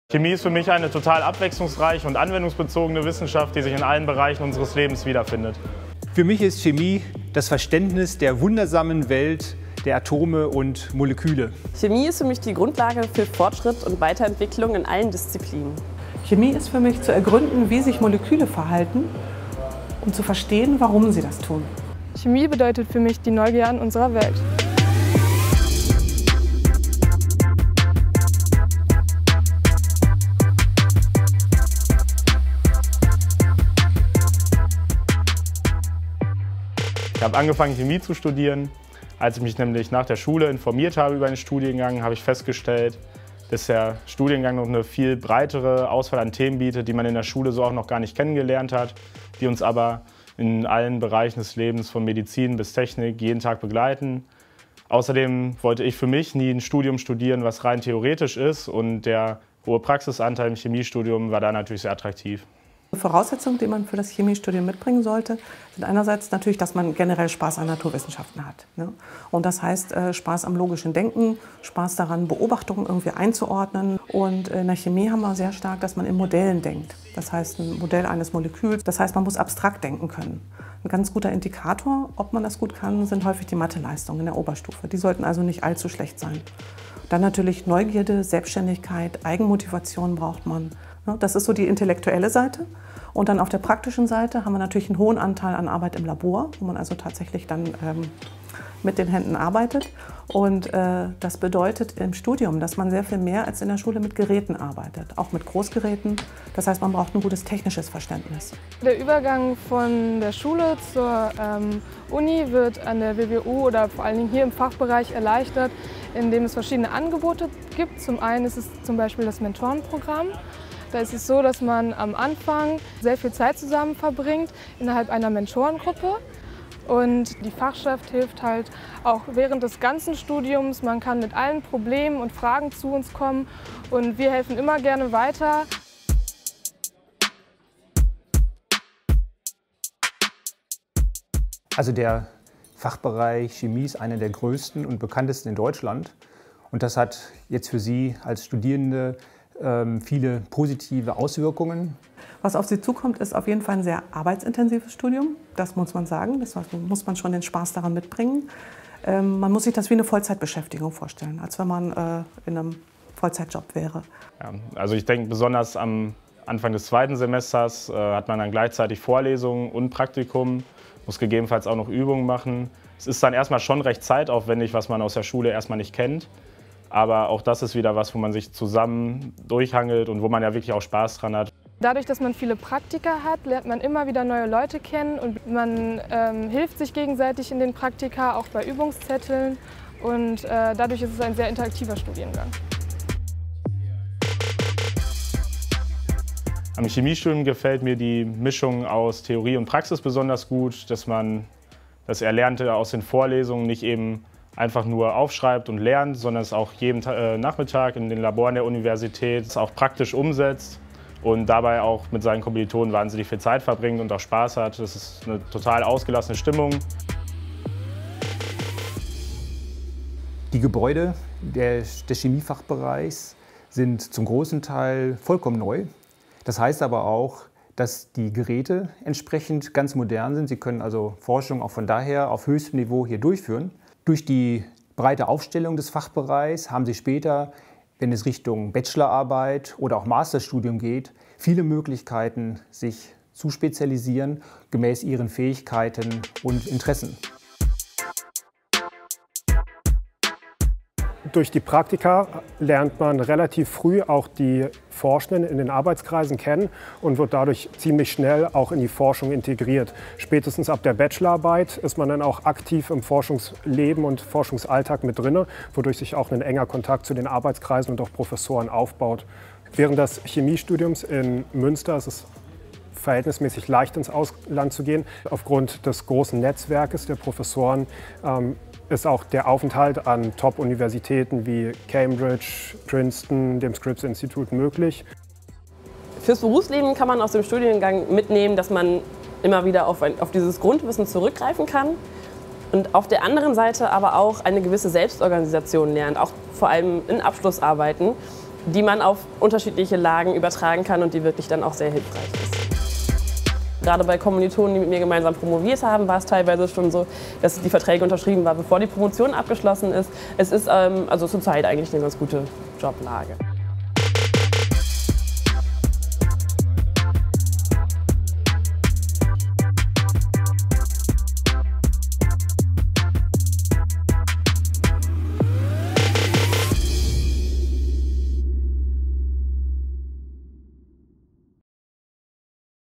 Die Welt der Atome und Moleküle verstehen – das Chemiestudium in Münster
Chemie findet sich in vielen Bereichen unseres Lebens wieder. Das ergründet man im Chemiestudium an der WWU nicht nur in der Theorie, sondern auch in der Praxis. Forschendes Lernen steht hier an erster Stelle. Durch die breite Aufstellung des Fachbereichs wird ein umfangreiches Grundlagenwissen vermittelt, das Studierende auf das Masterstudium und eine mögliche Promotion vorbereitet.